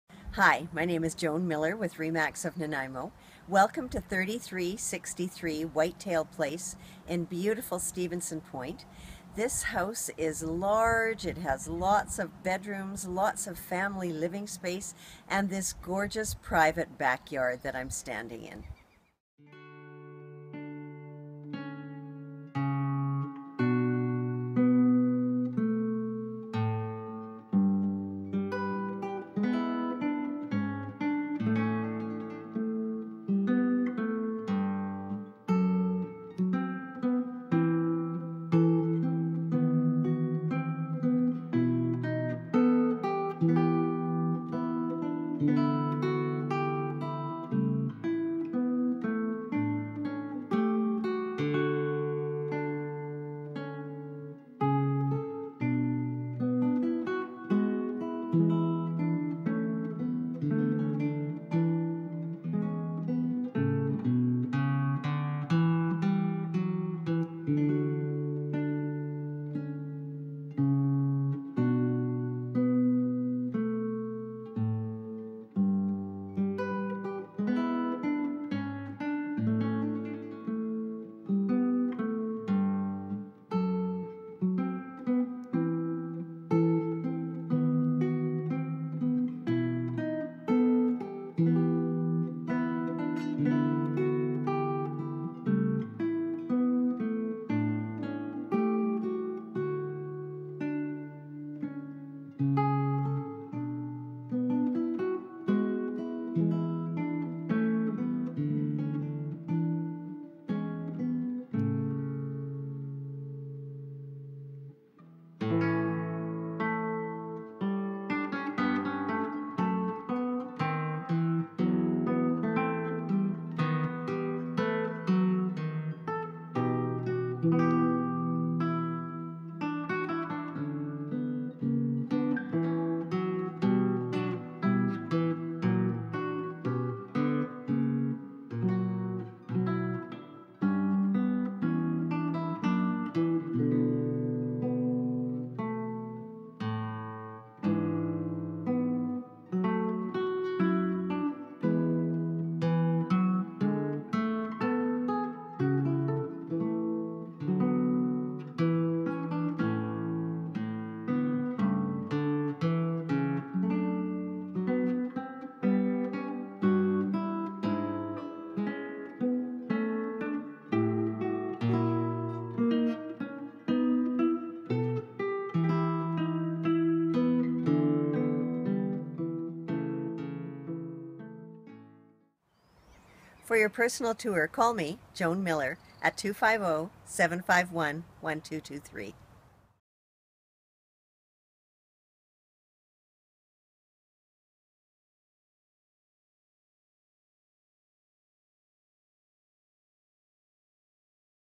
3363 Whitetail Place - Miller Real Estate Nanaimo - Airborne Video Photography
Situated on a quiet cul de sac of fine homes in a desirable Stephenson Point neighborhood is this beautiful and immaculate four bedroom residence.  Snuggled into its own paradise, this custom home was originally built as a showhome for Nanaimo’s Parade of Homes and takes full advantage of its unique West Coast landscape.  A separate driveway provides enough parking for two RV’s and an arbour welcomes you in to enjoy the side yard and lower patio.

Entry is through an oversized front door into a grand two storey foyer where a curved sky lit staircase leads to the main level. High ceilings, attractive trim, hardwood flooring, recessed lighting and a warm colour palette add elegance. The well-appointed living room with its vaulted ceiling and large windows adjoins the dining room where your guests will dine in style. Wood flooring extends into the kitchen which is every cook’s dream.  Warm cabinetry is abundant and a center island with eating bar adds even more work space.  A built-in wine rack, cookbook shelves, planning desk, and pantry area ensures everything is in its place.  Three stainless appliances and a high end hood fan have recently been installed and will stay with the house.  A window seat with extra storage is a perfect spot to converse with the cook. The kitchen is open to the family room with its cozy gas fireplace.

One of the finest features of this home is level access to a huge rear deck from the main level family room which is rare in this style of home.  Down the hall are two spacious bedrooms and a beautiful principal bathroom with linen closet.  The master bedroom boasts an attractive stepped ceiling, walk-in closet and luxurious ensuite with soaker tub and separate shower. Glass doors fill the room with views of the natural landscape and offer access to the deck where a morning coffee is a must.

The lower level is fully finished providing even more space for a busy family.  There are two spacious bedrooms, a full bathroom, large laundry room, hobby area and sizeable family room with access to a charming lower patio. Adjacent to the front door is a den with 11’ ceilings.

The outdoors truly is an extension of the home.  At the rear, this sun dappled sanctuary keeps the integrity of its natural setting in mind.  A grove of Gary Oak and Arbutus trees combine with pretty camas plants for a low maintenance yet striking backdrop to the expansive deck.  Whether relaxing on long summer days, or barbecuing for friends, this oasis is akin to having your home and recreational property in one.

Added features of this special home include durable hardi plank siding, alarm system, built-in vac, irrigation system, and newer hot water tank.  A new furnace and heat pump with allergy filters was installed just over a year ago. This superb location is within minutes of beaches, fitness center and ferries.  Welcome to your dream home.

Joan Miller - RE/MAX of Nanaimo